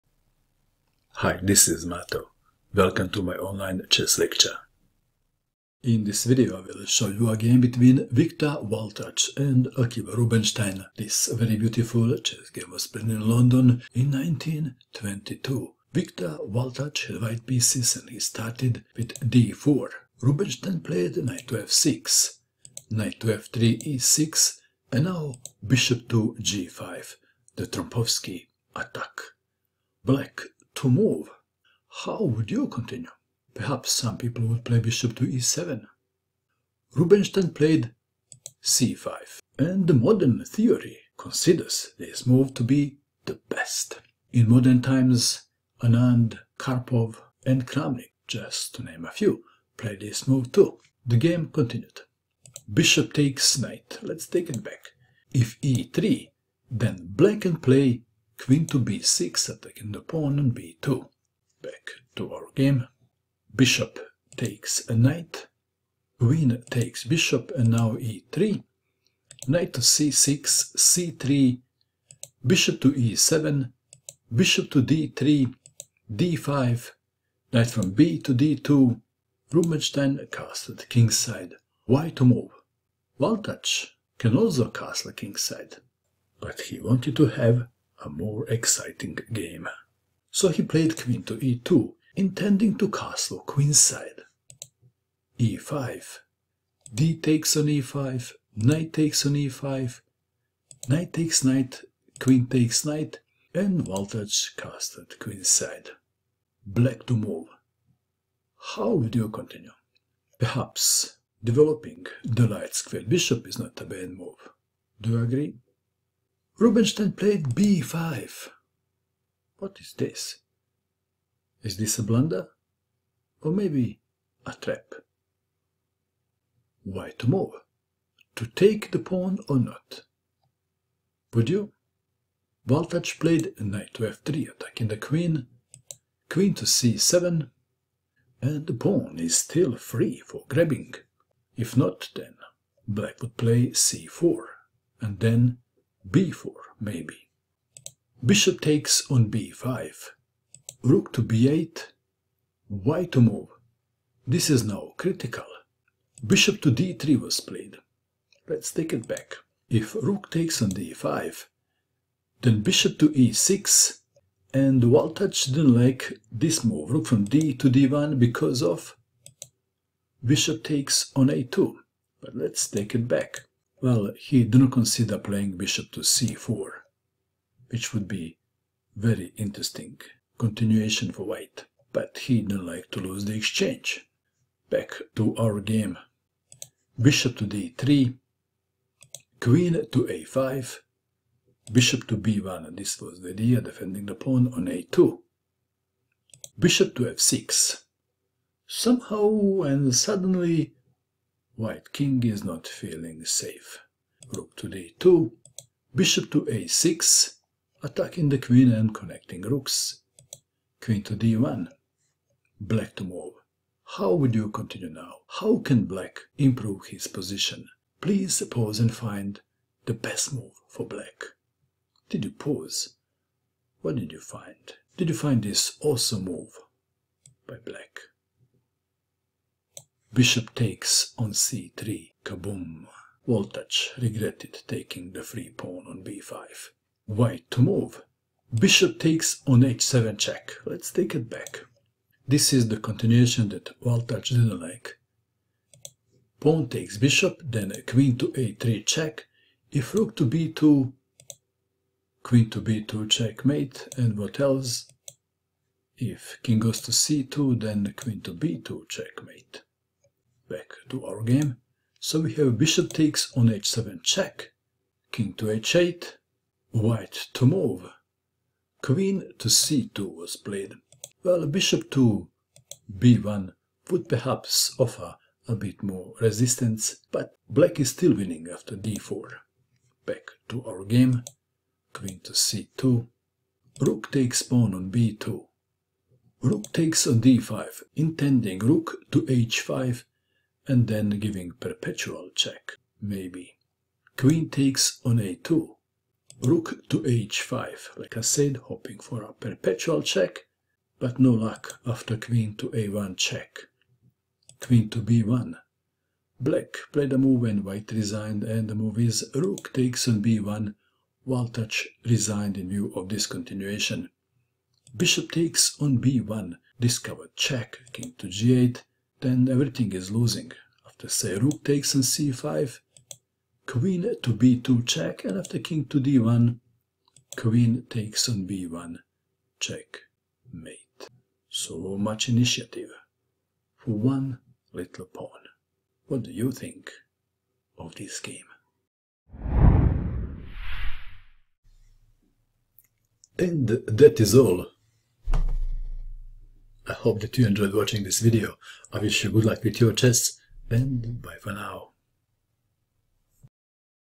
Sacrifice A Pawn And Call The Shots | Victor Leonard Wahltuch vs Akiba Rubinstein: London 1922
Charles G M Watson vs Akiba Rubinstein
London 1922, London ENG, rd 5, Aug-05
Indian Game: Knights Variation. General (A46)  ·  0-1
1. d4 Nf6 2. Nf3 e6 3. Nbd2 c5 4. e3 Nc6 5. Be2 b6 6. O-O Bb7 7. b3 Be7 8. Bb2
O-O 9. Ne5 Qc7 10. Bf3 d5 11. Rc1 Nxe5 12. dxe5 Nd7 13. c4 Nxe5 14. cxd5 exd5
15. Qc2 Nxf3+ 16. gxf3 f5 17. Kh1 Bf6 18. Rg1 Rae8 19. Rg3 f4 20. Rh3 g6 21.
Rg1 Bxb2 22. Qxb2 Qg7 23. Qc2 fxe3 24. fxe3 Rxe3 25. Rhg3 Rf6 26. Rf1 Re2 27.
Qd3 Rfe6 28. Ne4 Rxa2 29. Nc3 Rb2 30. Nd1 Ree2 31. f4 c4 32. Qf5 gxf5 0-1